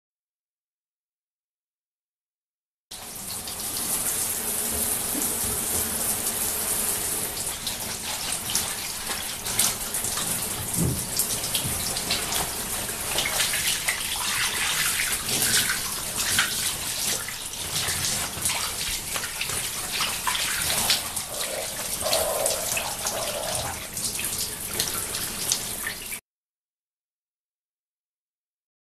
Showering and Bathing Sound Effect ♪
Bathing in the shower sound effect. The sound of someone taking a shower.